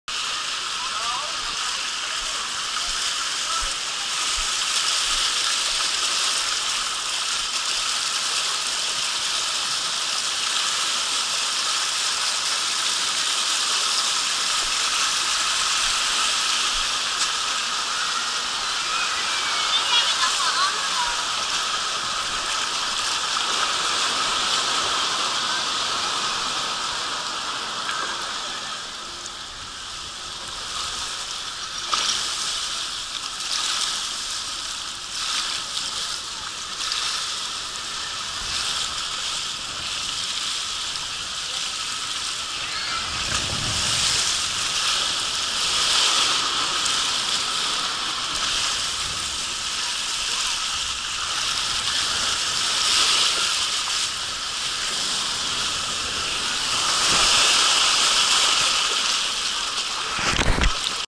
White Whirlwinds Water Slide at Splash Works
White Whirlwinds water park slide at Splash Works water park at Canada's Wonderland in Vaughan, Toronto, Ontario, Canada.